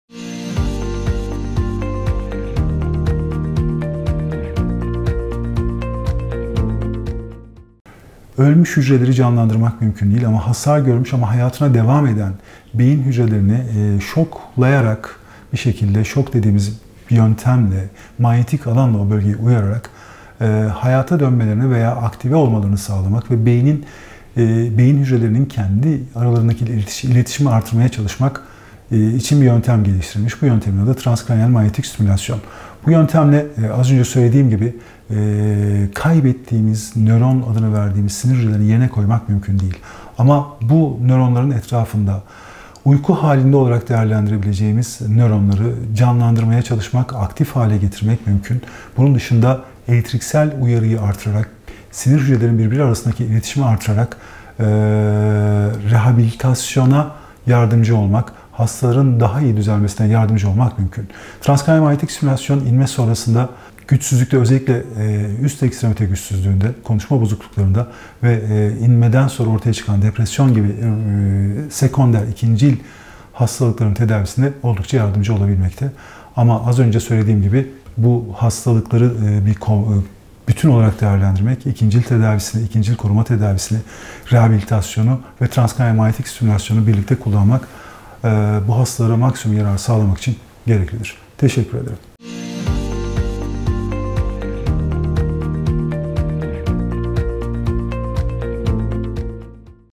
İnme tedavisinde transkranyal manyetik Stimülasyon (TMS) nedir? - Prof. Dr. Talip Asil
Memorial Hizmet Hastanesi Nöroloji - Girişimsel Nöroloji Bölümü'nden Prof. Dr. Talip Asil, inme tedavisinde transkranyal manyetik Stimülasyon (TMS) nedir? sorusuna yanıt verdi.